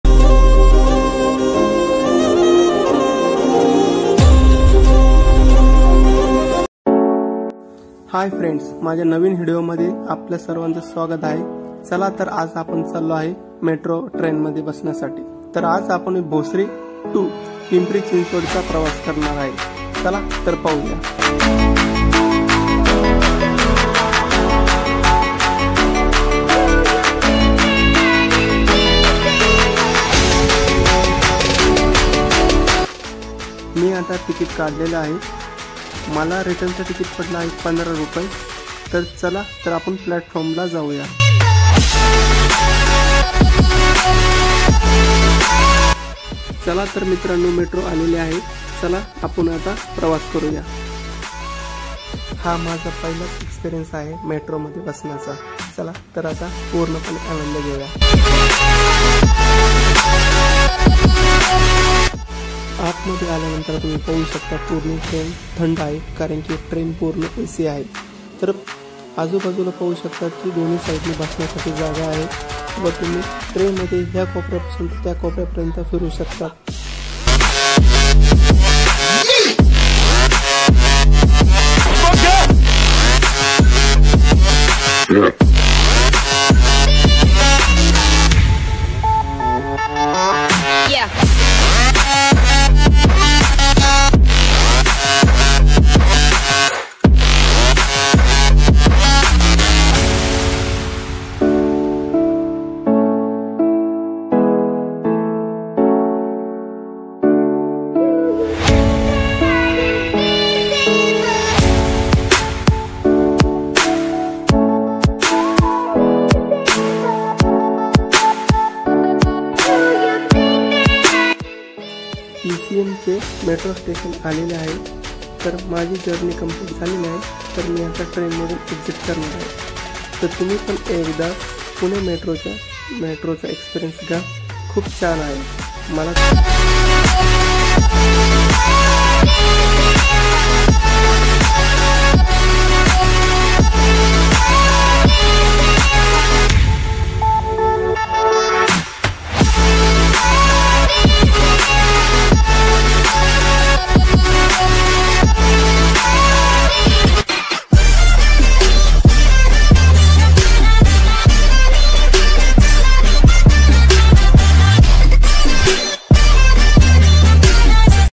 Pune Metro | Our Experience in Pune Metro | Bhosari to PCMC Metro | Pune Metro Vlog
Recently we traveled through Pune Metro. This is a newly launched Pune metro and we are sharing our experience. Kindly share this vlog with your family and friends.

When Pune Metro started?